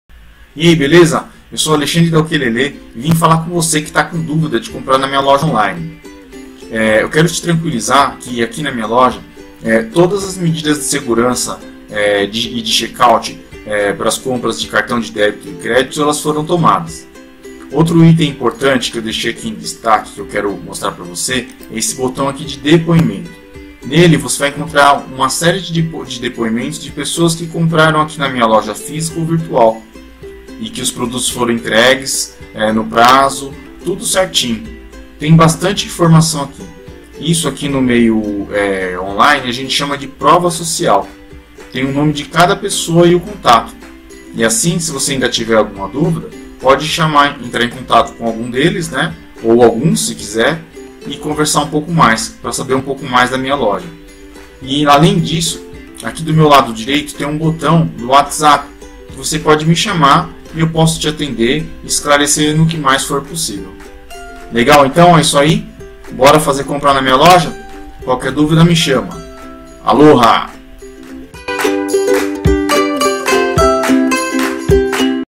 Página de depoimentos de Clientes da Ukelele
Olá, meu nome é Alexandre da Ukelele, fiz este vídeo para você que está com dúvida de comprar na minha loja on line. Quero te tranquilizar que todas as medidas de segurança com relação ao check out e informação do cartão de crédito foram tomadas. Também coloque uma página com o depoimento de diversos clientes que compraram na loja aqui comigo, todo mês a lista de clientes é atualizada. Isto é uma “prova social” que deixei aqui para você, caso queira entrar em contato e se certificar de mais alguma coisa. No site também tem um botão do Whatsapp caso queira falar diretamente comigo.
Aloha!